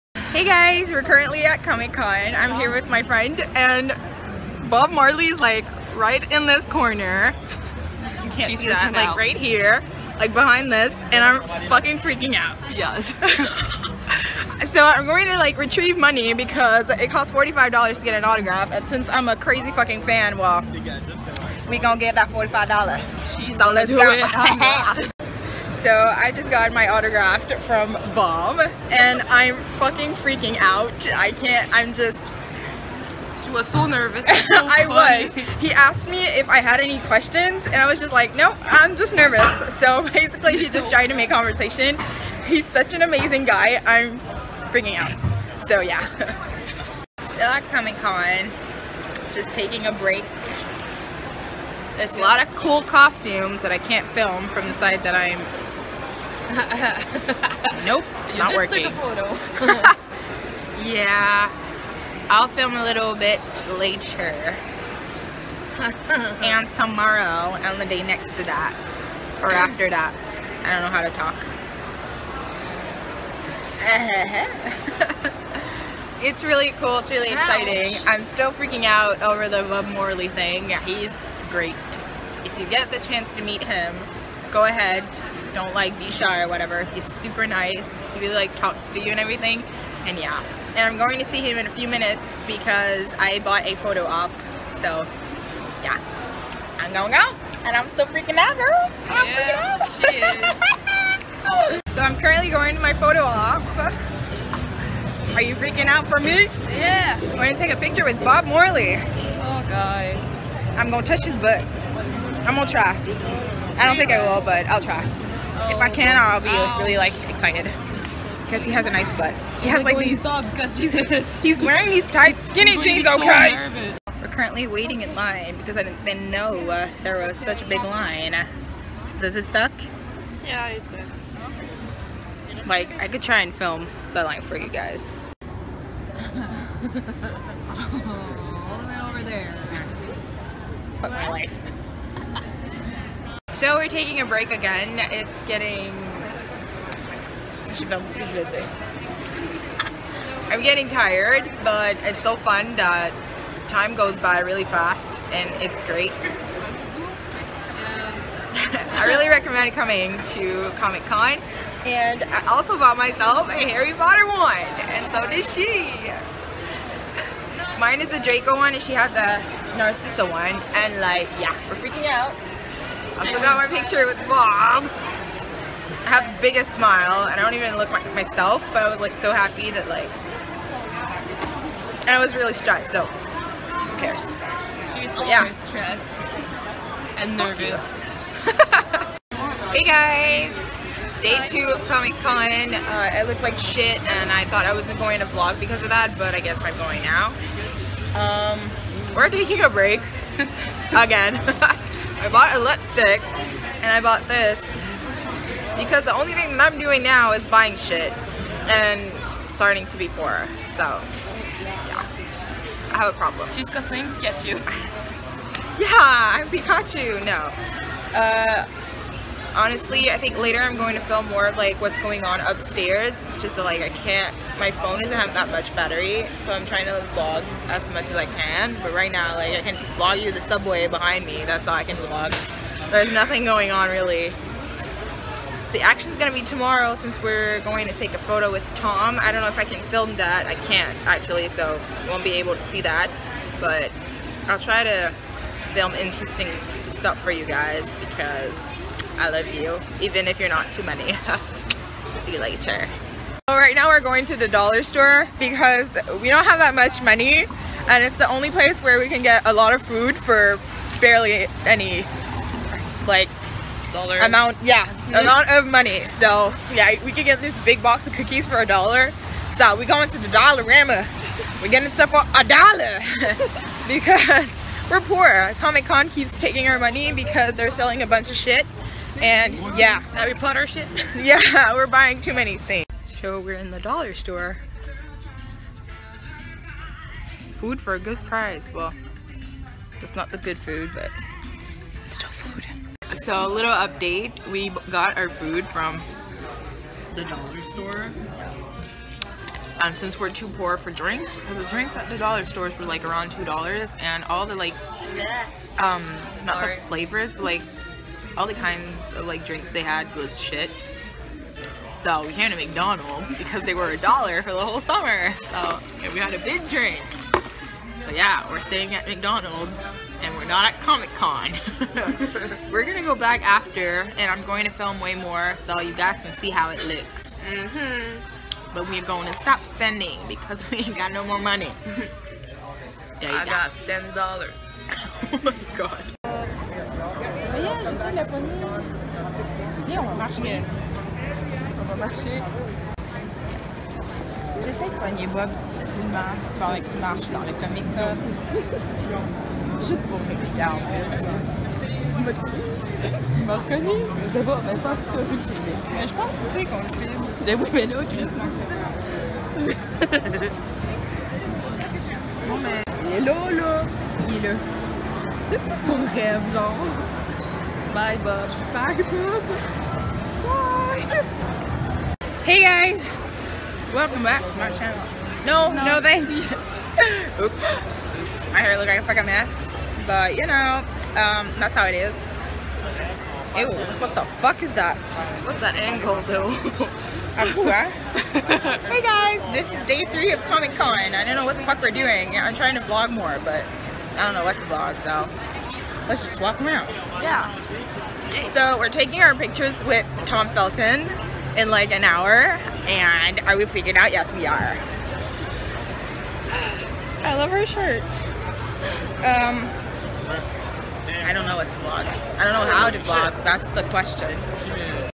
STALKING BOB MORLEY AT COMIC CON | First Vlog ! (Comic Con Montreal 2016)
» Tried to vlog but it's really hard to get my camera out when I'm trying to focus on having fun ! Also pretty hard to film the cool parts with Bob Morley et Tom since we weren't aloud to film/take pictures of the celebrities there. :(  Also sorry I haven't posted in a while, I have been without internet for the past week so I'll try to make it up to you !! xx Love you, don't forget to like, comment and subscribe xoxoxoxox